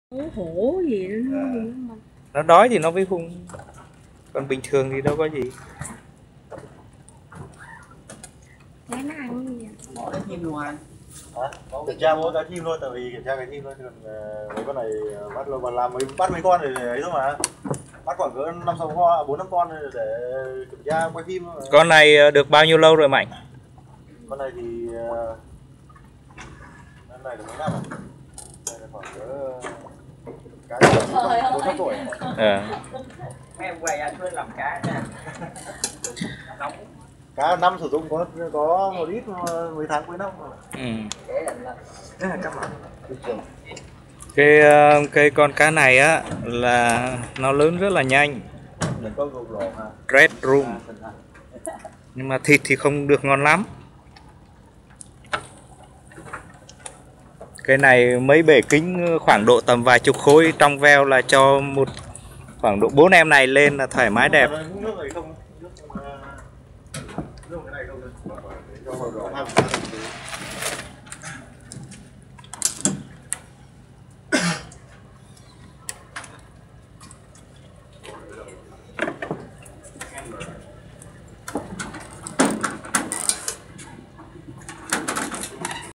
AquaFish 2017
Red drum broodstock cultured at Nha Trang bay